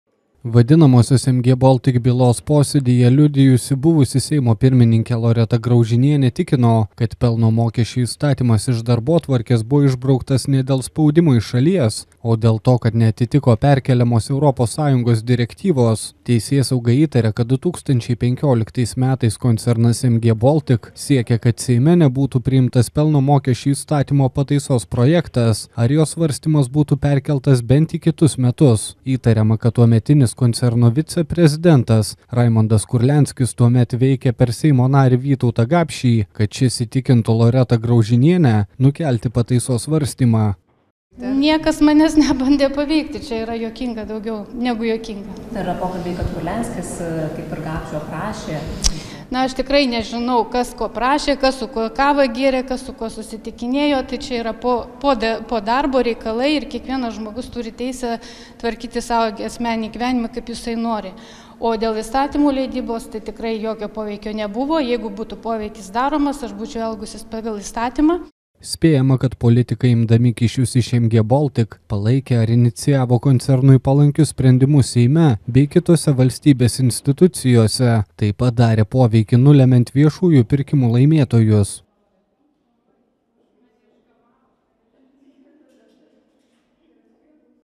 L. Graužinienė teisme prabilo apie „MG Baltic“ dominusį įstatymą
Vadinamosios „MG Baltic“ bylos posėdyje liudijusi buvusi Seimo pirmininkė Loreta Graužinienė tikino, kad Pelno mokesčio įstatymas iš darbotvarkės buvo išbrauktas ne dėl spaudimo iš šalies, o dėl to, kad neatitiko perkeliamos Europos Sąjungos direktyvos.